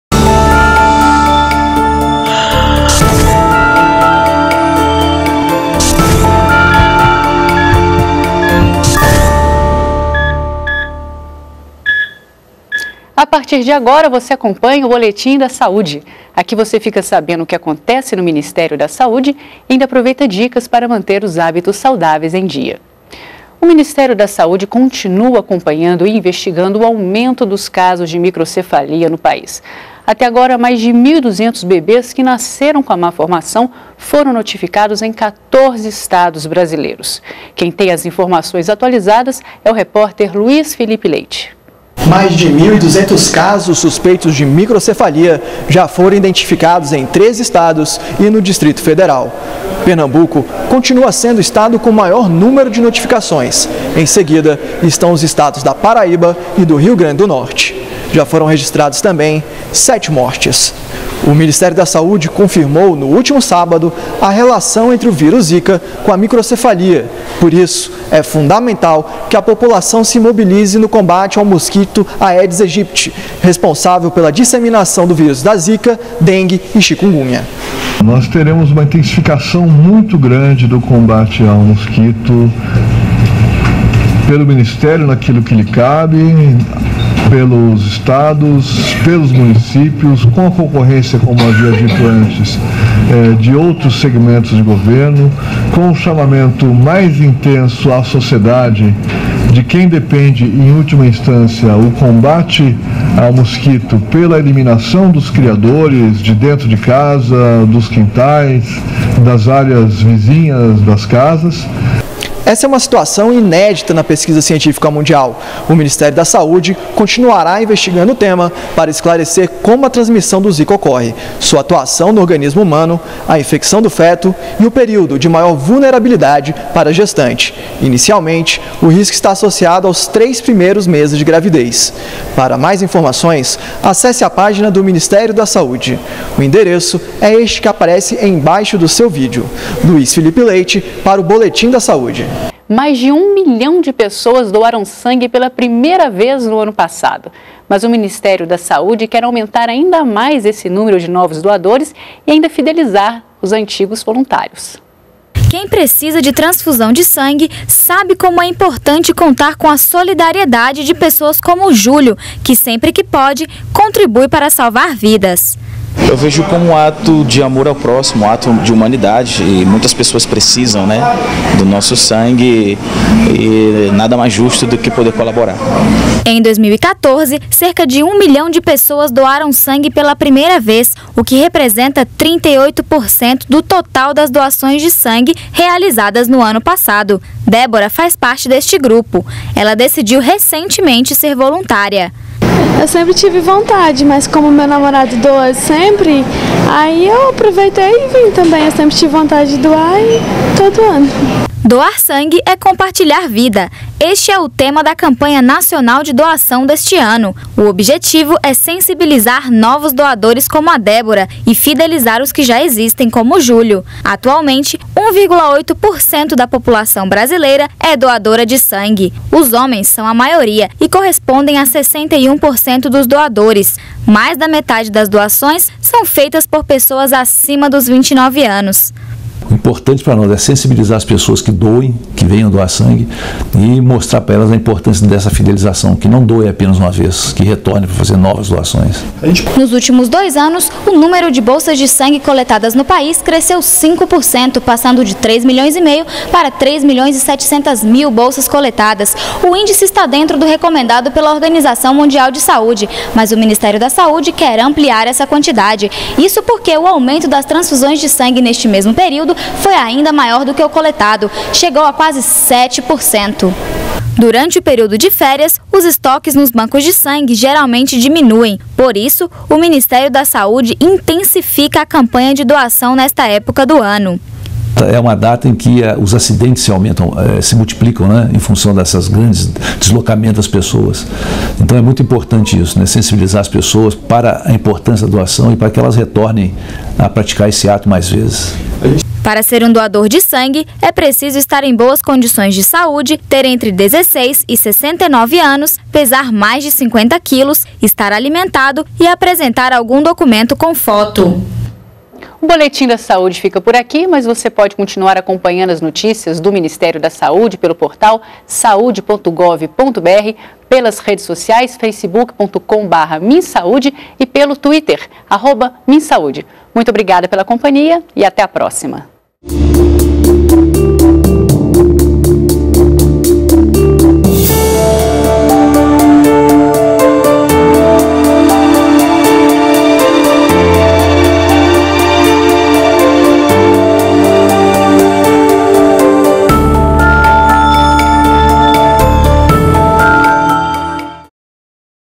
Boletim da Saúde - 4 de dezembro
BOLETIM DA SAÚDE - 04.12.15: Mais de 1,2 mil casos de microcefalia já foram identificados em 13 estados e no Distrito Federal. Ministério da Saúde continua a investigar o tema. No ano passado, cerca de 1 milhão de pessoas doaram sangue pela primeira vez - Ministério quer aumentar esse número.